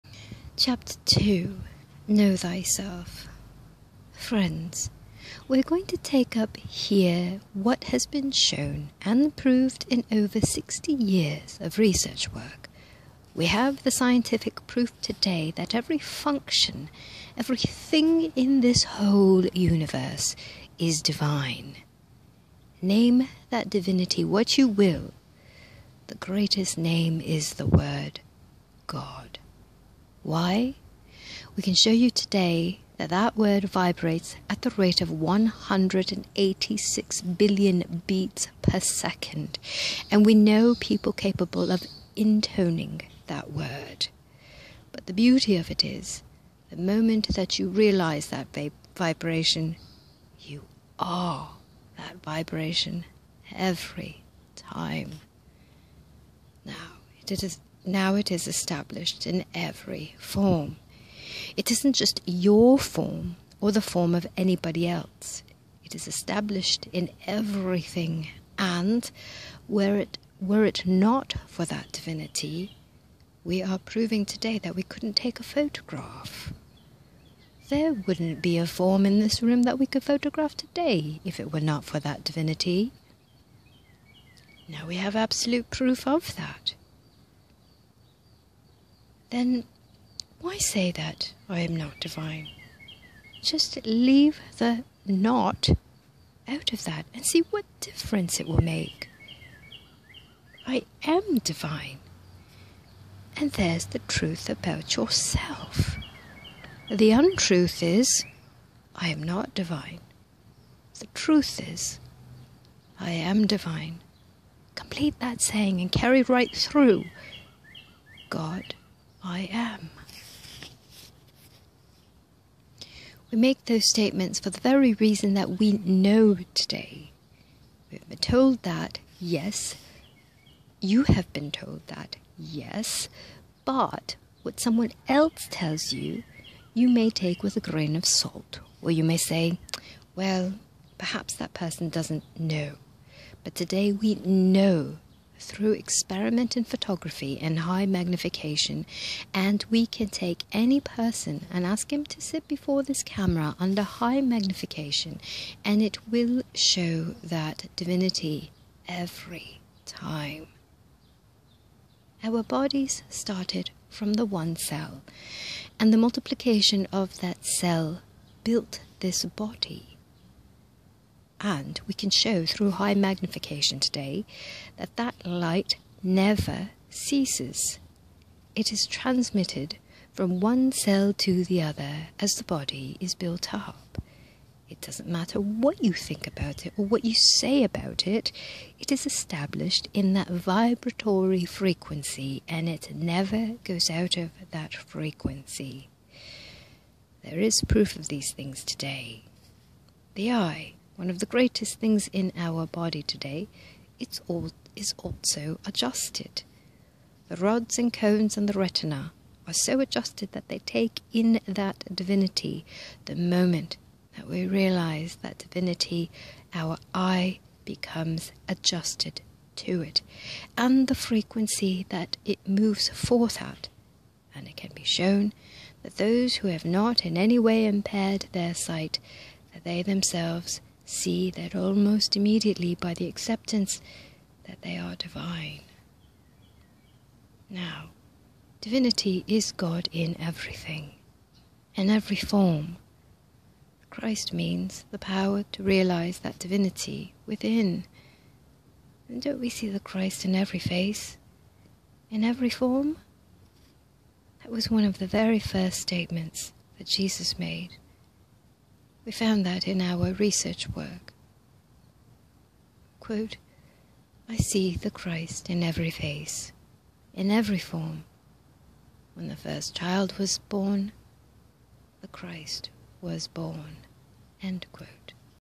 Ch2 (1of 4)- Life & Teaching of the Masters of the Far East: KNOW THYSELF
.."...Our bodies started from the one cell and the multiplication of that cell built this body, and we can show, through high magnification today, that that light never ceases. It is transmitted from one cell to the other as this body is built up. It doesn't matter what you think about it or what you say about it, it is established in that vibratory frequency and it never goes out of that frequency..." (P.28)

With great love~
xxM